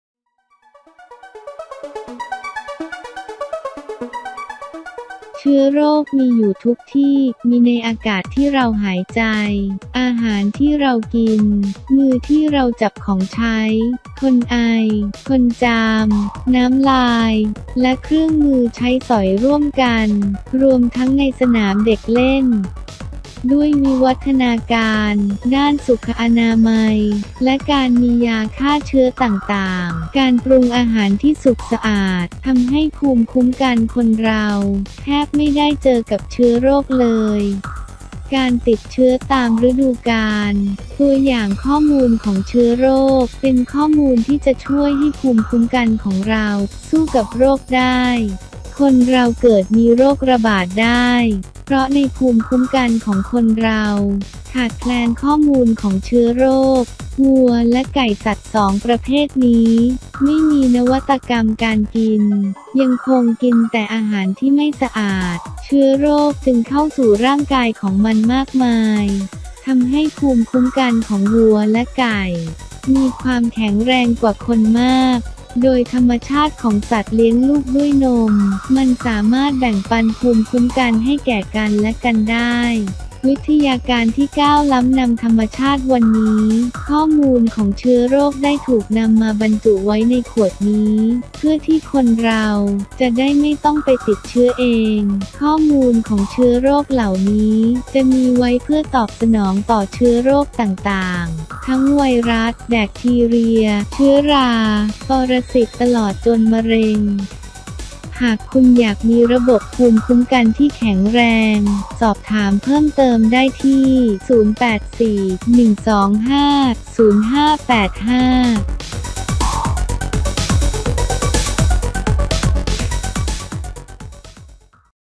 รู้หรือยัง วัวและไก่ แบ่งปัน ภูมิคุ้มกัน ให้คนเราได้ หนึ่งในความหวัง ต่อต้านโรคระบาด
4ไล้ฟ์ ทรานสเฟอร์ แฟกเตอร์  โทร. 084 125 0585  เชื้อโรคมีอยู่ทุกที่. มีในอากาศที่เราหายใจ อาหารที่เรากิน มือที่เราจับของใช้
คนไอ คนจาม น้ำลาย เครื่องมือใช้สอยร่วมกันรวมทั้งในสนามเด็กเล่น  ด้วยวิวัฒนาการ ด้านสุขอนามัย และการมียาฆ่าเชื้อต่างๆการปรุงอาหารที่สอาด ทำให้ภูมิคุ้มกันคนเราแทบไม่ได้เจอเชื้อโรคเลย

การติดเชื้อตามฤดูกาล ตัวอย่างของเชื้อโรค เป็นข้อมูลช่วยให้ภูมิคุ้มกันของเรา สู้กับโรคได้คนมีโรคระบาด เพราะในภูมิคุ้มกันของคนเรา ขาดแคลนข้อมูลของเชื้อโรค

วัวและไก่ สัตว์ 2 ประเภทนี้ ไม่มีนวตกรรมการกินยังคงกินแต่อาหารไม่สะอาด เชื้อโรคจึงเข้าสู่ร่างกายของมันมากมาย ภูมิคุ้มกันของวัวและแก่ จึงมีความแช็งแรง โดยธรรมชาติของสัตว์เลี้ยงลูกด้วยนม มันแบ่งปันภูมิคุ้มกัน ให้แก่กันได้

วิทยาการที่ก้าวล้ำนำธรรมชาติ วันนี้ ขอมูลของเชื้อโรคถูกนำมาบรรจุไว้ในขวดนี้ เพื่อที่คนเราจะไม่ต้องไปติดเชื้อเอง ข้อมูลของเชื้อโรค มีไว้ตอบสนอง เชื้อโรคต่างๆ ทั้งไวรัส แบกทีเรีย เชื้อรา ปรสิต ตลอดจนมะเร็ง

โทรสอบถามก่อน สายด่วน : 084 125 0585